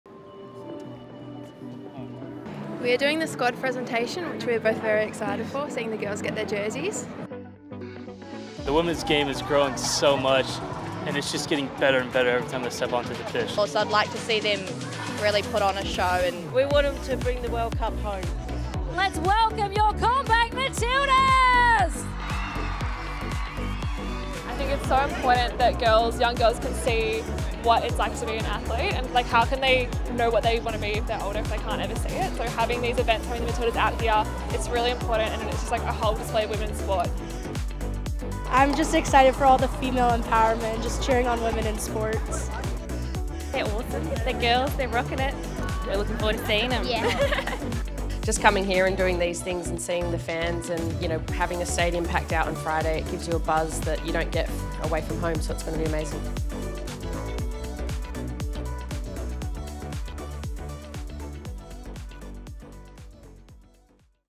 Behind the scenes of yesterday's Fed Square Final Squad Presentation
Yesterday at Fed Square, the squad was presented with their jerseys by 23 people named Matilda.